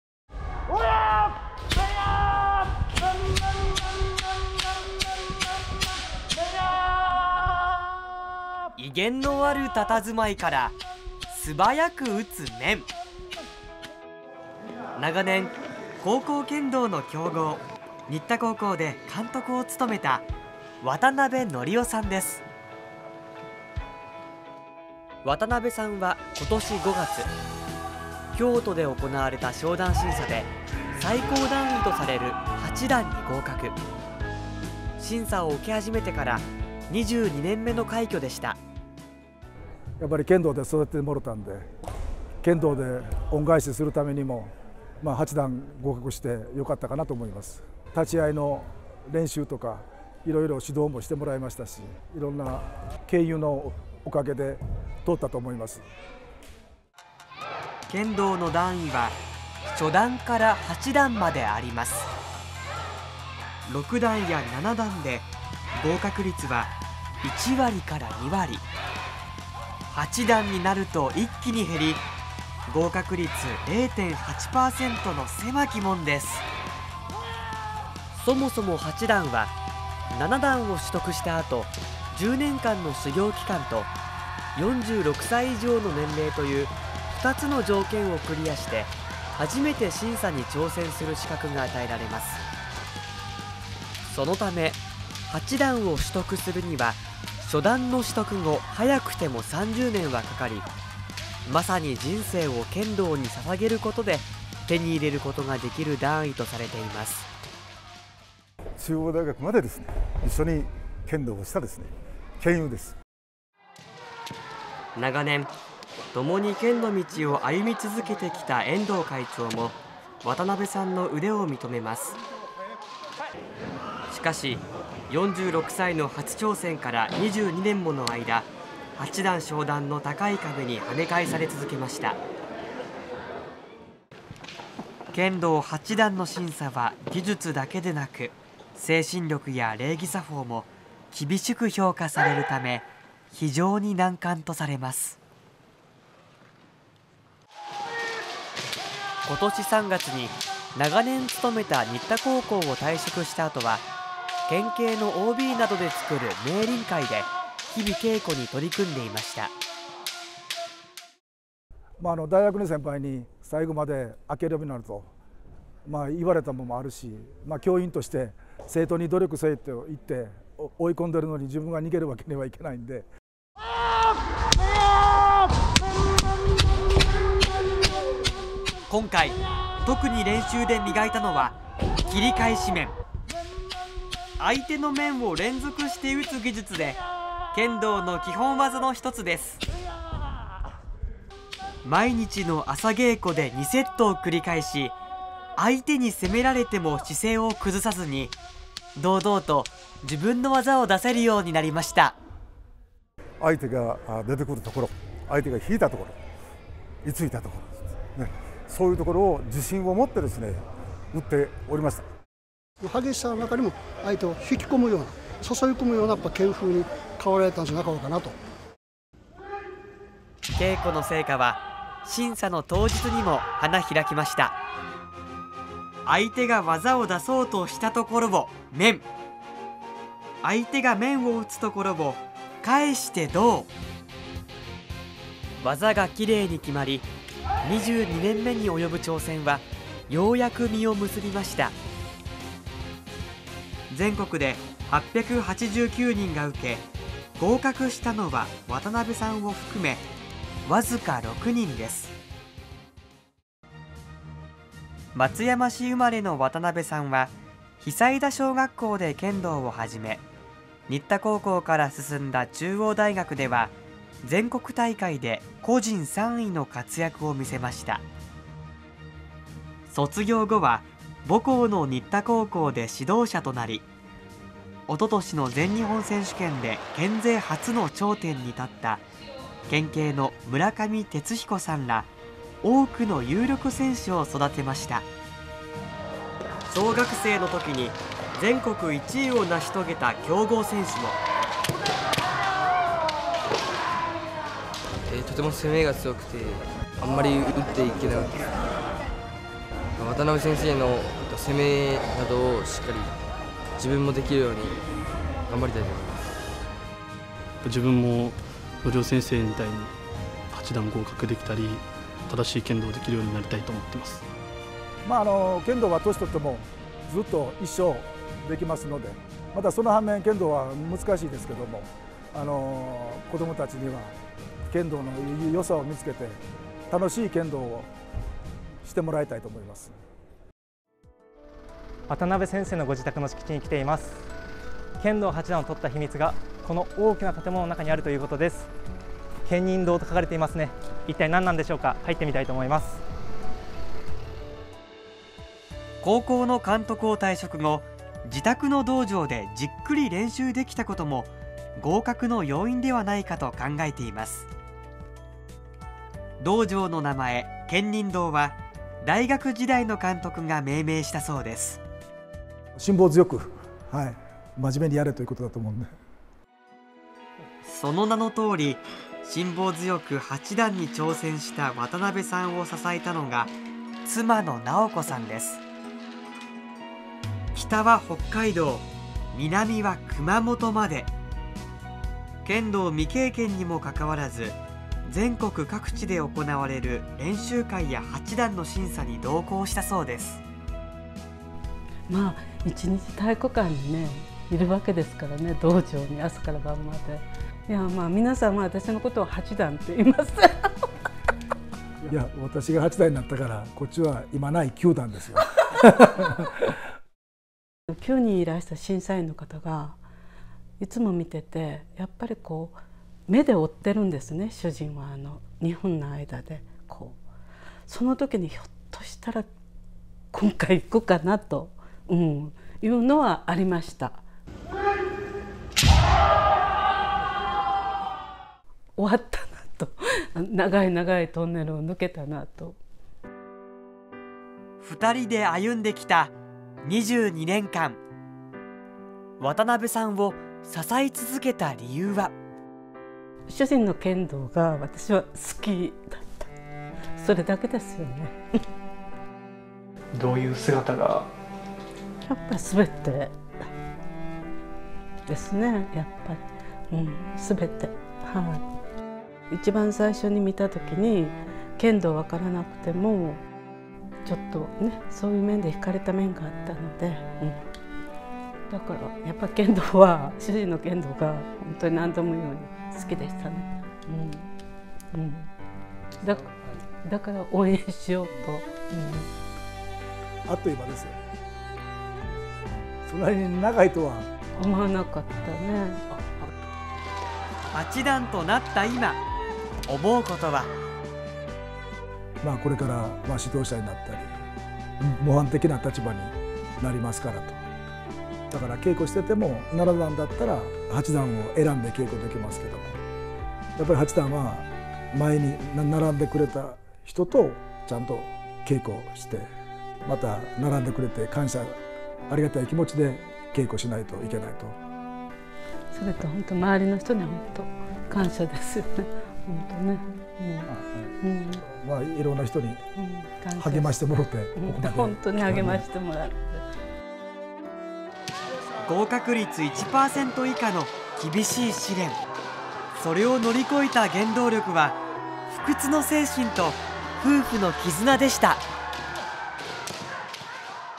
【剣道】0.8％の合格率「主人の剣道が私は好きだった」２２年間、夫婦で挑んだ剣道八段への道 渡部憲雄さん
日本の伝統的な武道『剣道』
その「国内最難関」の審査に22年間挑み続け、今年5月、ついに合格率1％以下とされる八段に合格した男性が松山にいます。夫婦二人三脚でつかみ取った段位です。

2024年7月23日放送